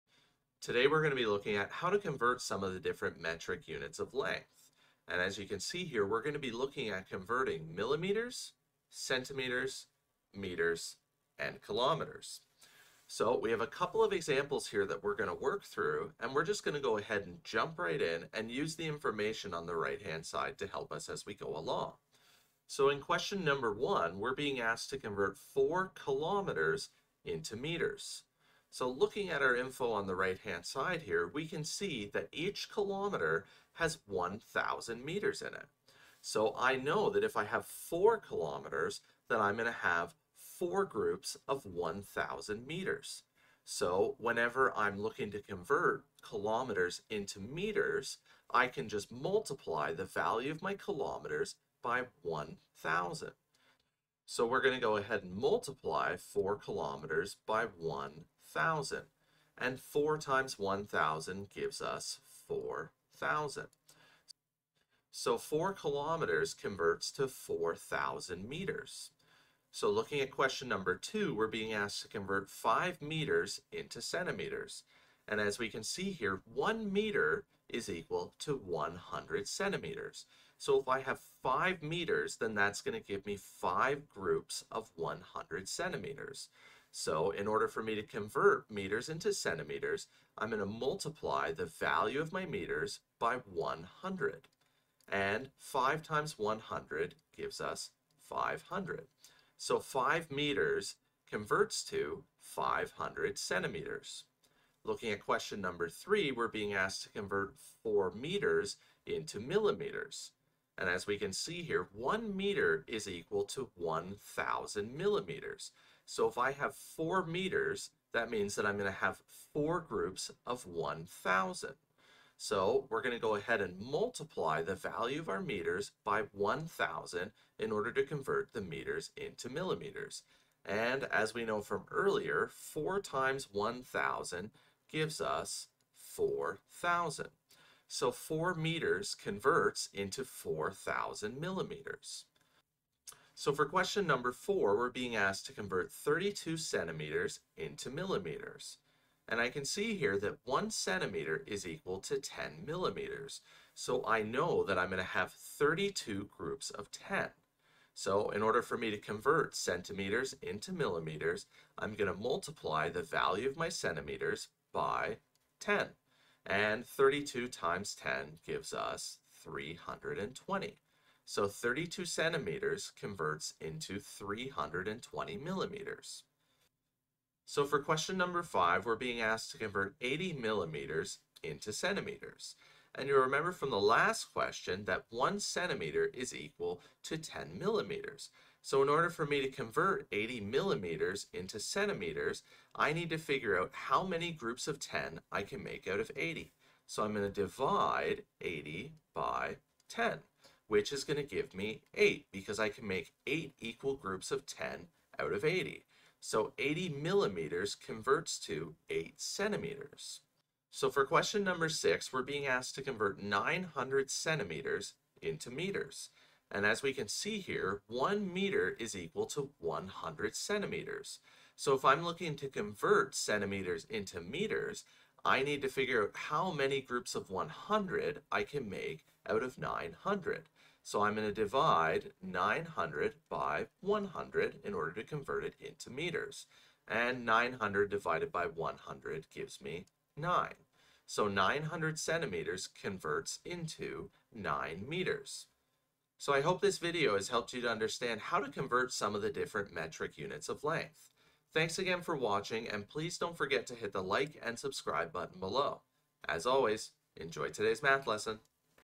Easily Convert Metric Units Of Length | How to Convert Millimeters, Centimeters, Meters & Kilometers
How do I convert some of the metric units of length? In today's episode, we explore the steps involved in converting metric units of length.

This video includes a step-by-step for:
- how to convert km to m
- how to convert m to cm
- how to convert m to mm
- how to convert cm to mm
- how to convert mm to cm
- how to convert cm to m

Math can be complicated sometimes. Here at Uncomplicated Math, we strive to make mathematics less complicated.